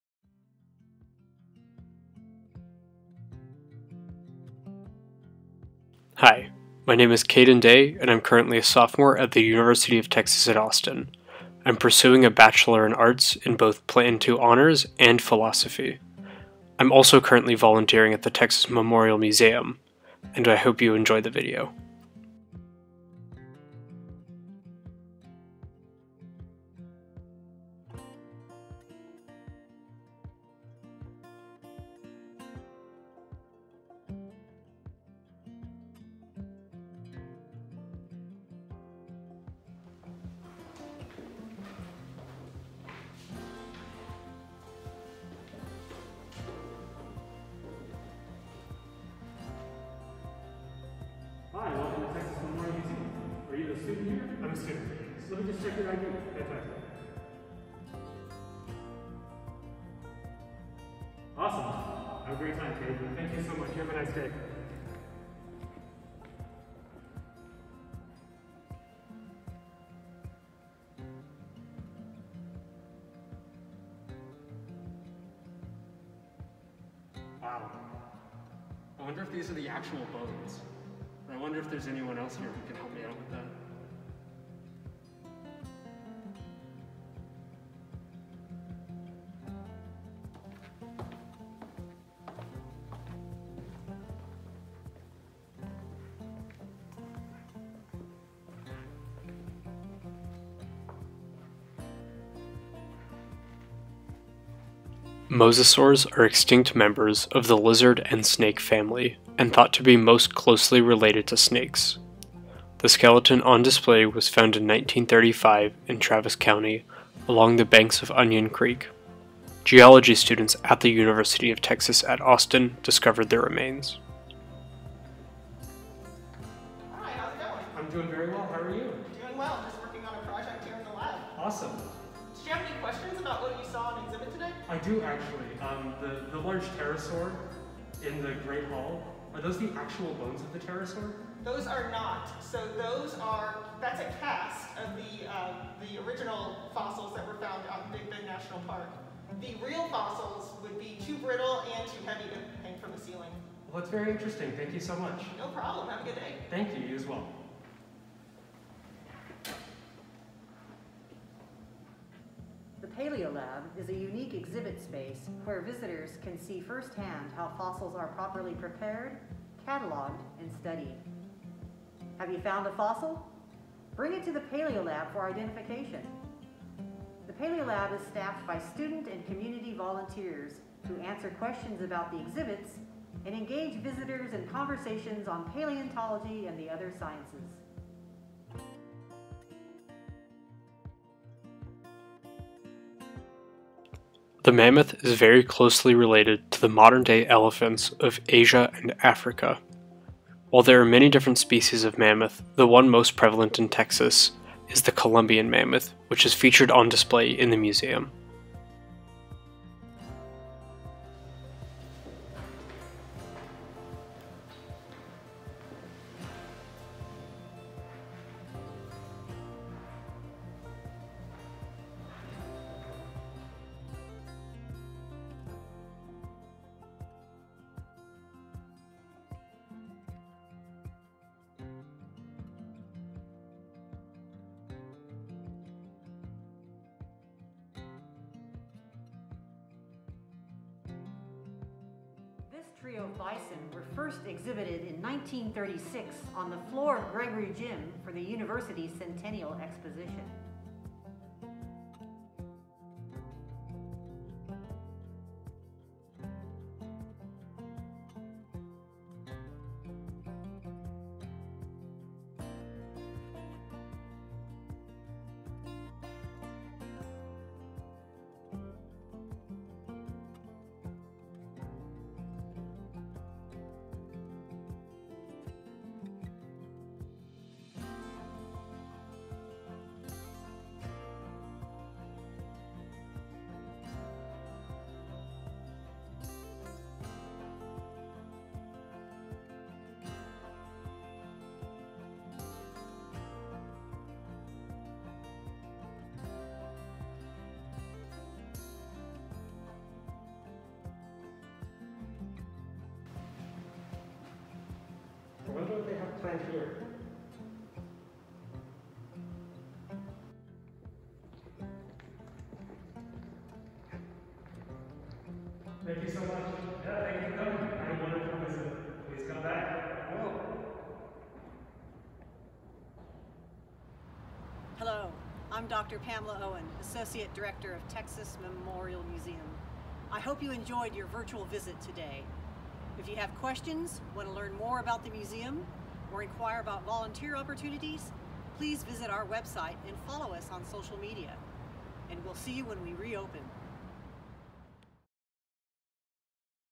ARCHIVE- Discover Texas Memorial Museum (2021)
Explore Texas Memorial Museum through the eyes of a UT undergraduate student. See creatures from Texas' prehistoric past and exhibits that highlight wildlife in the natural regions of Texas.